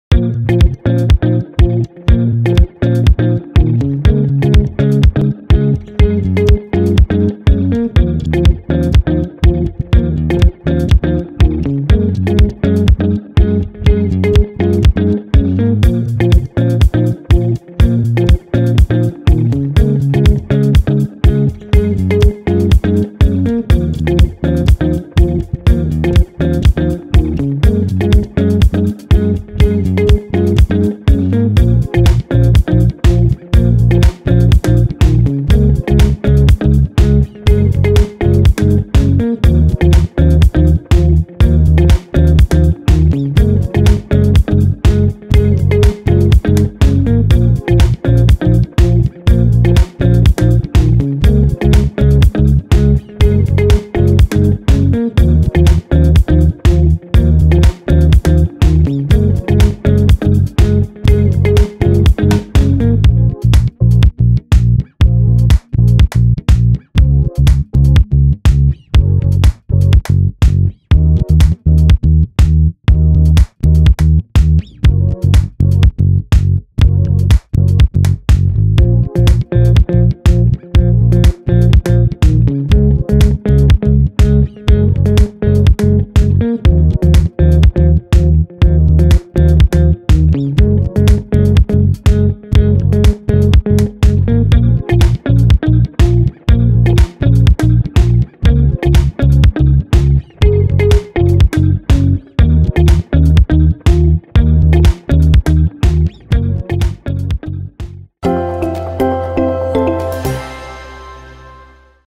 PLUGGAGE TUTORIAL - alert & carousel modes
This video explains you how to set up the alert mode and the carousel mode on your smart suitcase PLUGGAGE.